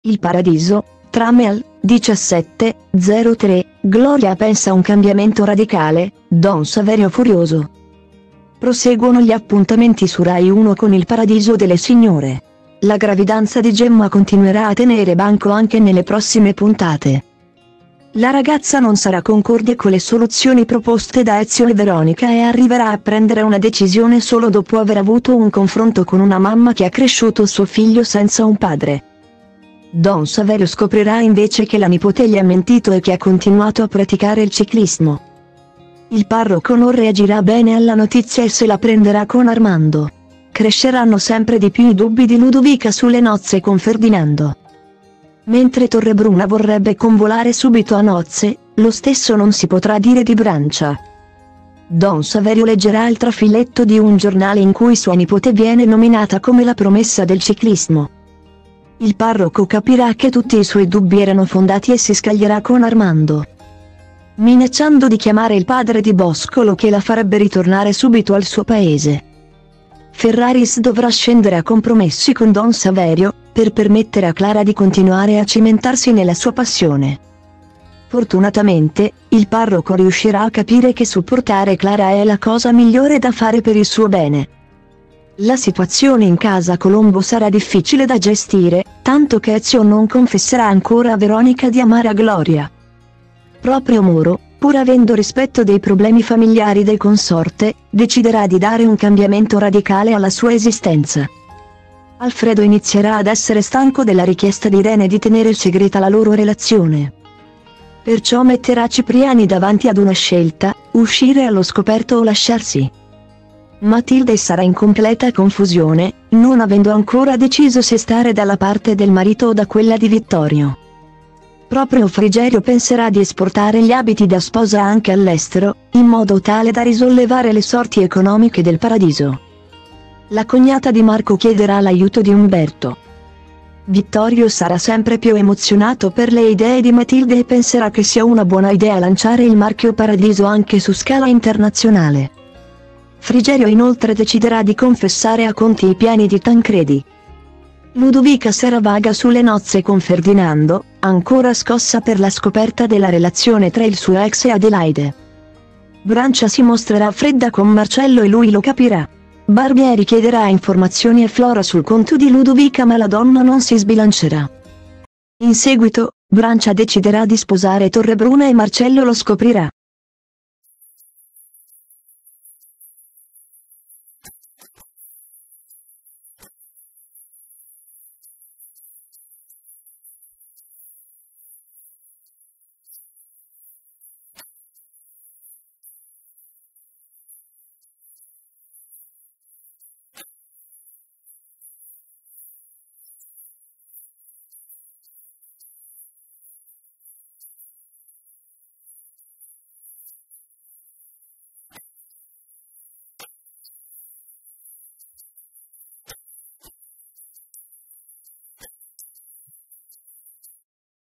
Il Paradiso, trame al 17/03: Gloria pensa un cambiamento radicale, Don Saverio furioso
Proseguono gli appuntamenti su Rai 1 con Il Paradiso delle Signore. La gravidanza di Gemma continuerà a tenere banco anche nelle prossime puntate.La ragazza non sarà concorde con le soluzioni proposte da Ezio e Veronica e arriverà a prendere una decisione solo dopo aver avuto un confronto con una mamma che ha cresciuto suo figlio senza un padre.Don Saverio scoprirà invece che la nipote gli ha mentito e che ha continuato a praticare il ciclismo.